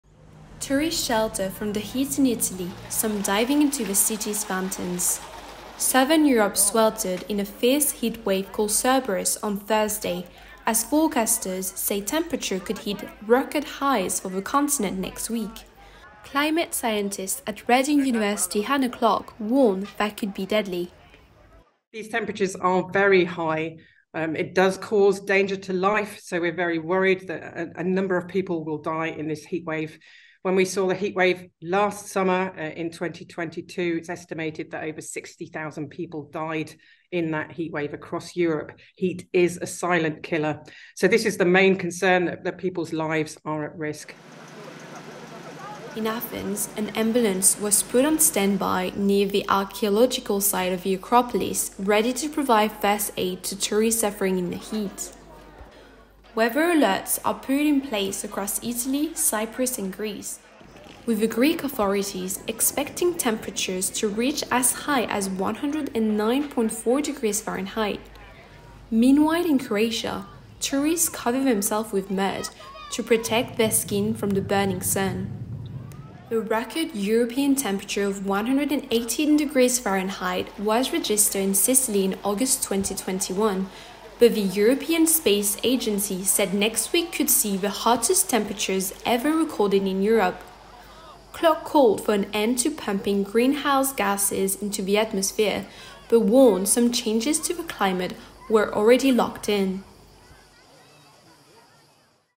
'Silent killer' warning as southern Europe swelters
Southern Europe is sweltering under a fierce heatwave with a warning that temperatures could hit record highs for the continent next week.